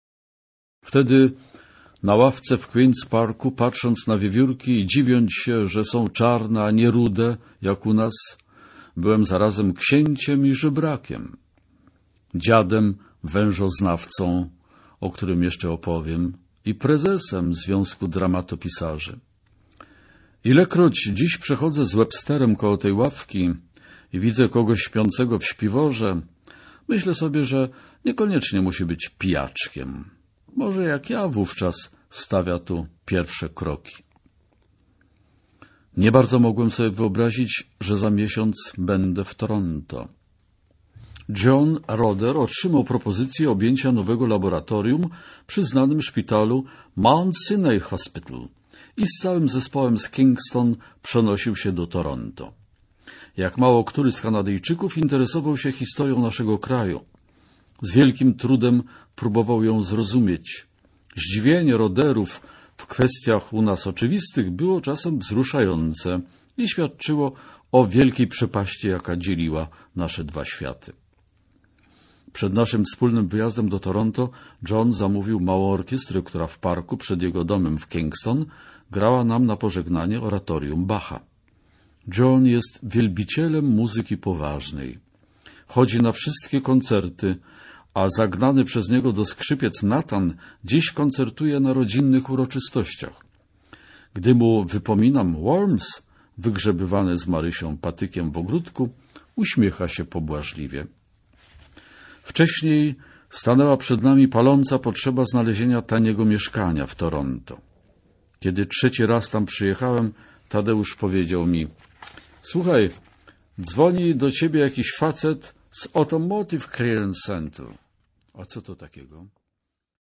Jarosław Abramow-Newerly - Młyn w piekarni MP3
Tylko u nas audiobooki Abramowa Newerly. Niezwykle barwni ludzie różnych narodowości spotykają się w polskiej piekarni w Toronto. Dużo humoru, ale też kawał gorzkiej prawdy o życiu Polaków w Kanadzie wypływającej z ich rozmów, kłótni i dyskusji.